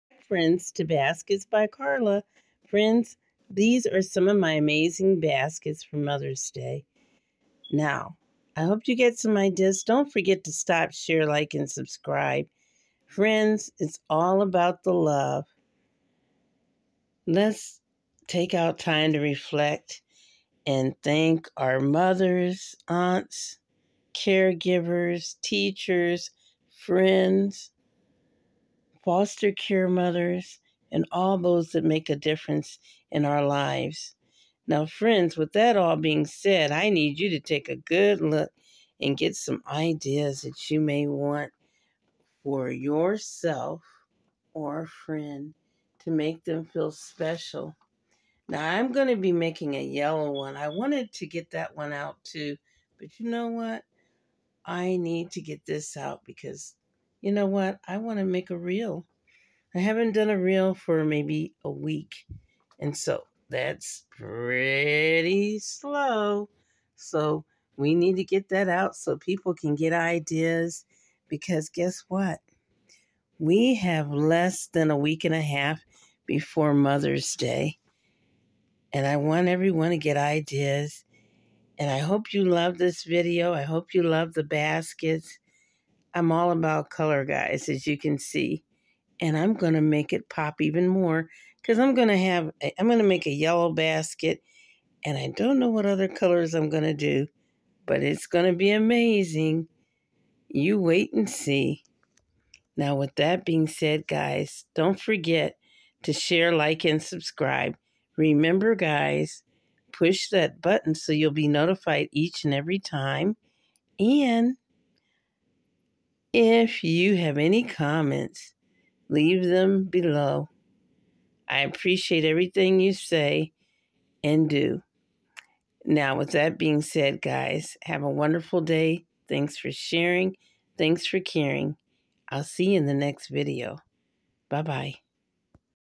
# Join Me In My Colorful World Of Baskets
Baskets of many sizes & colors:  aqua blue, peach, purple, beige, pink, blue and white ceramic, with puppies, bears , self care, etc...